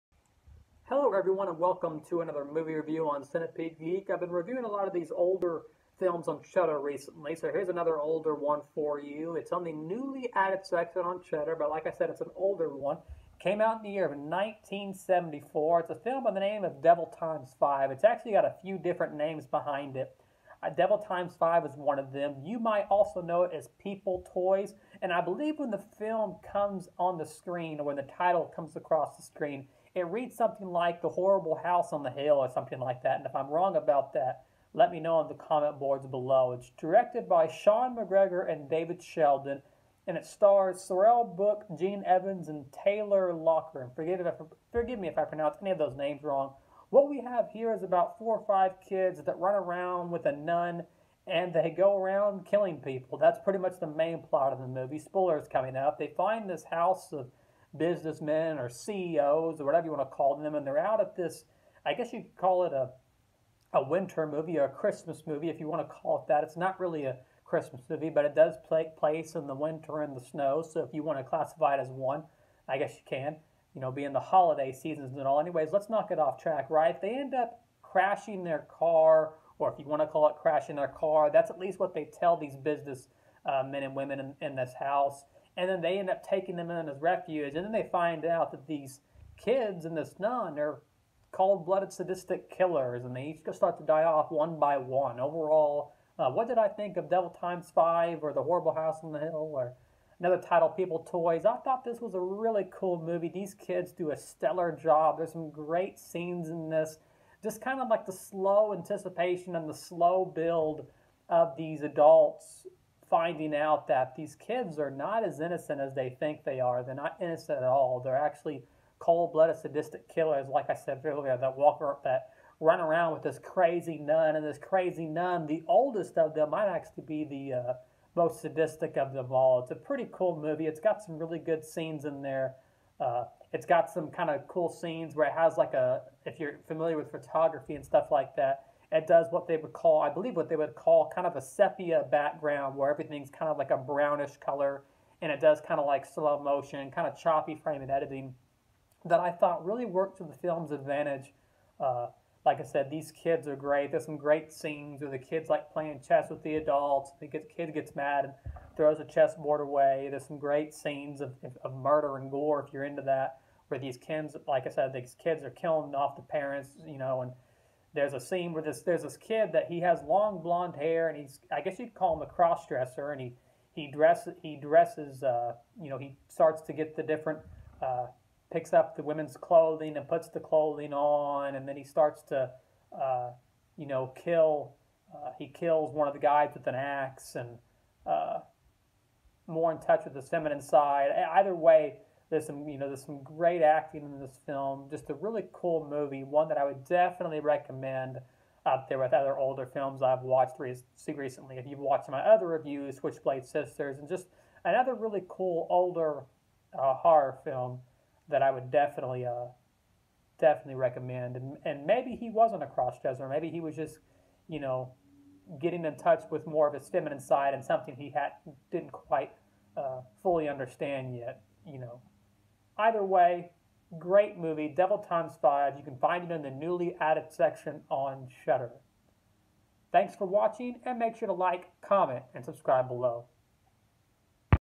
Devil Times Five Movie Review
In this video, I talk about a film called devil times five on shudder.